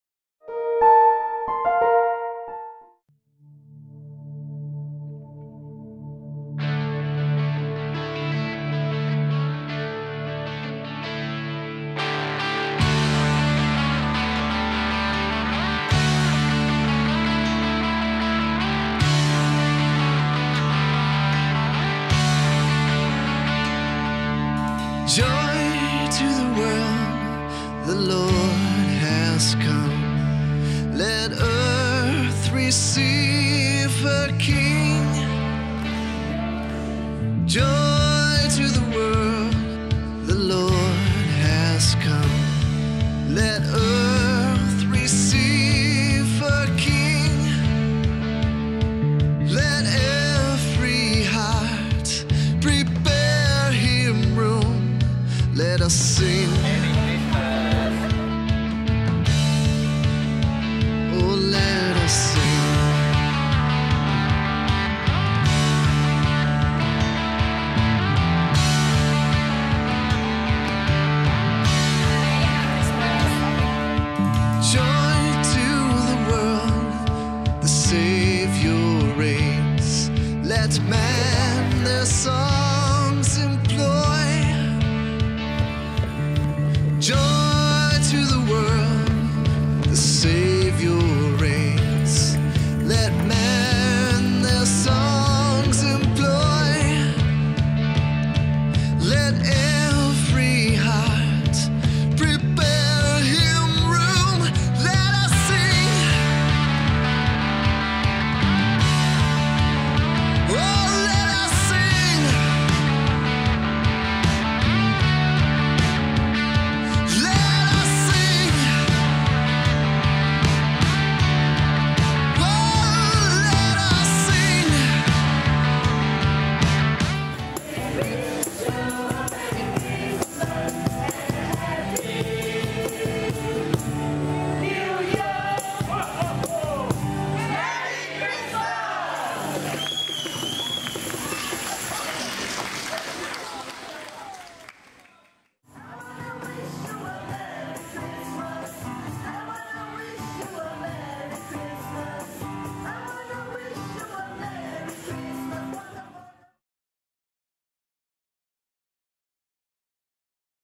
Bishop Bernard Paul's Christmas Open House 2019
An open house was held by His Lordship, Bishop Bernard Paul on Christmas day 2019.

Video produced by The Malacca Johore Diocese Office for Social Communications.